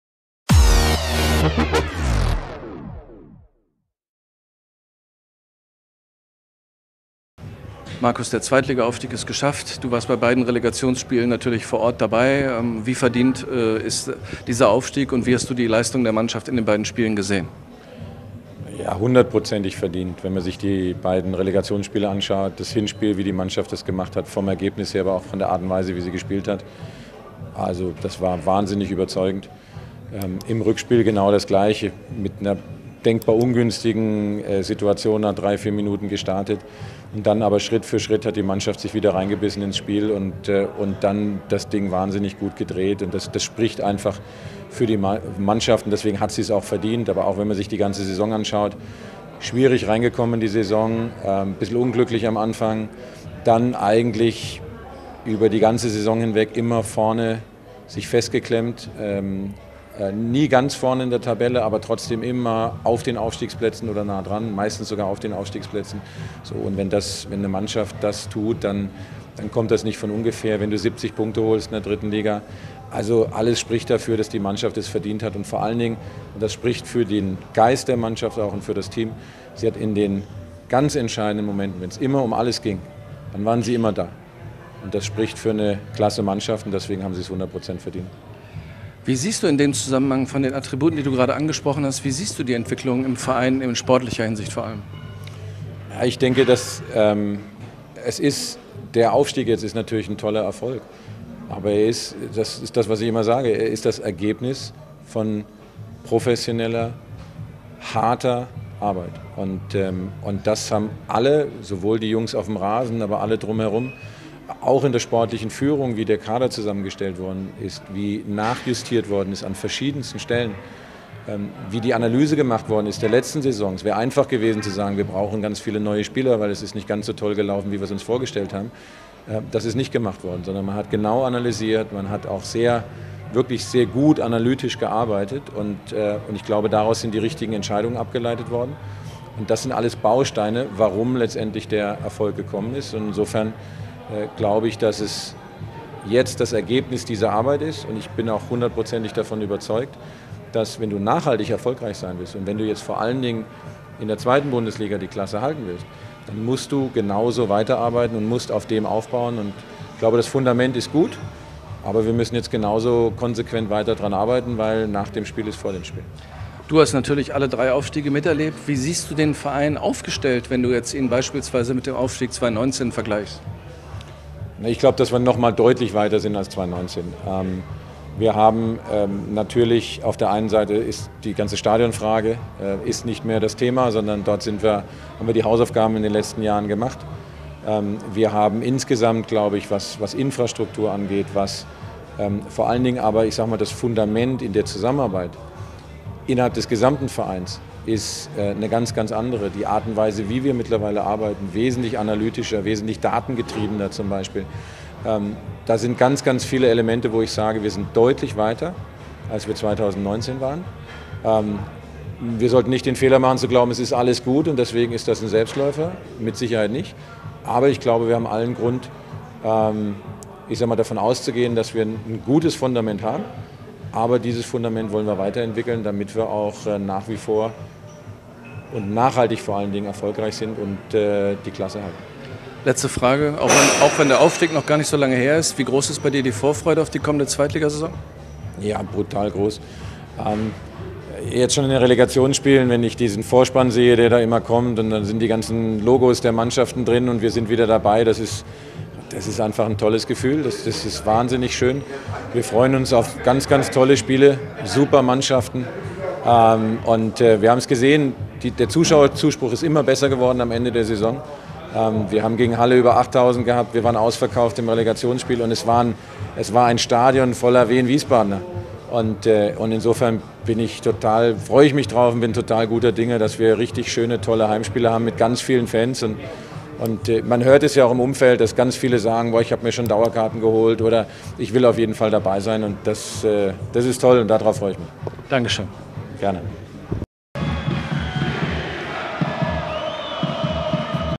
INTERVIEW I Markus Hankammer I SV Wehen Wiesbaden I Aufstieg 2. Bundesliga
Die Statements von Präsident Markus Hankammer beim Saisonabschluss nach dem Aufstieg in die 2. Bundesliga. [präsentiert von Mobilebet]